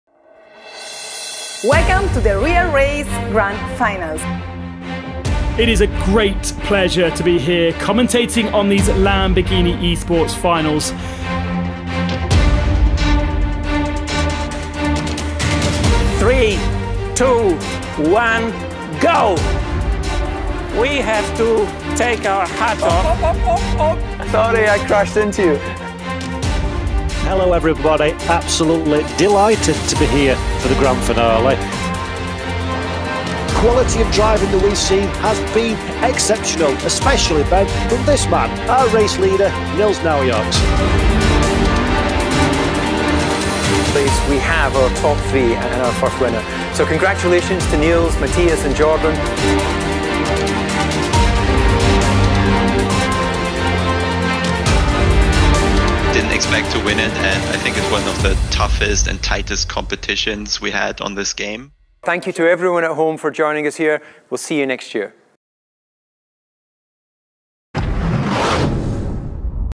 Lamborghini Esports 2020 highlights 👍
The race of a lifetime opens doors to experiences of a lifetime. Our first ever Esports competition was not only a huge success in every sense of the word but also exhilarating where the winner participated against 11 other drivers to emerge victorious. Relive the experience with us!